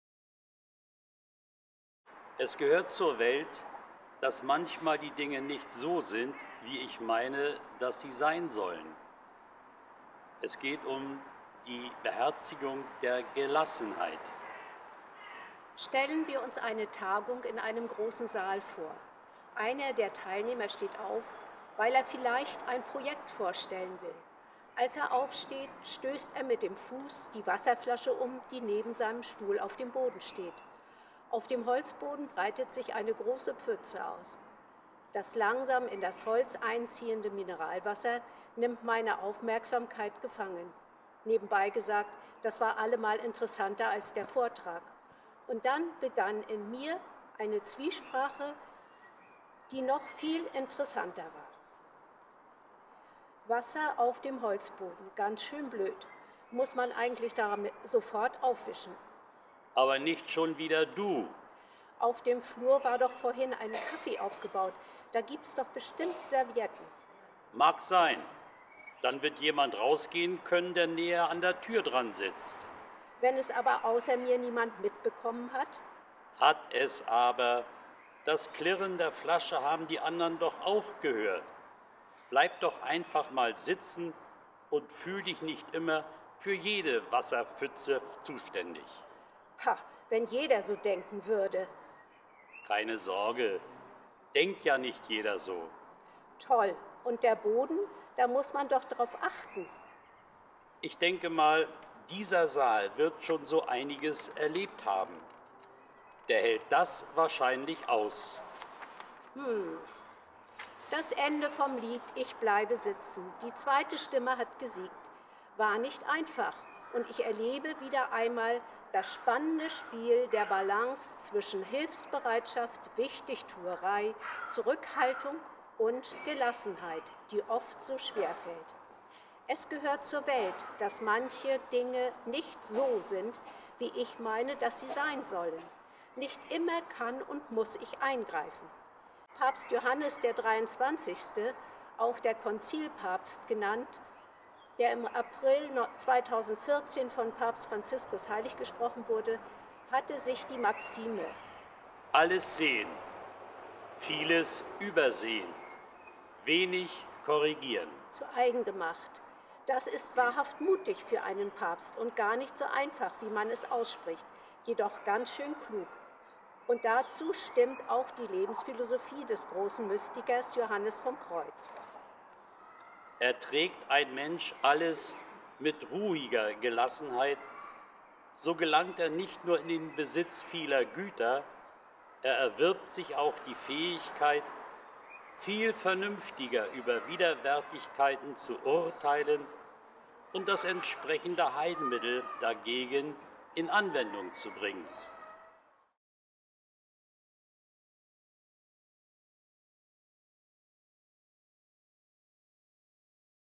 Fastenzeit 2021 – Die Beherzigung der Gelassenheit
In der Fastenzeit 2021 veröffentlicht die Gemeinde „Heilige Dreifaltigkeit“ Stralsund der katholischen Pfarrei St. Bernhard Stralsund-Rügen-Demmin täglich einen Impuls.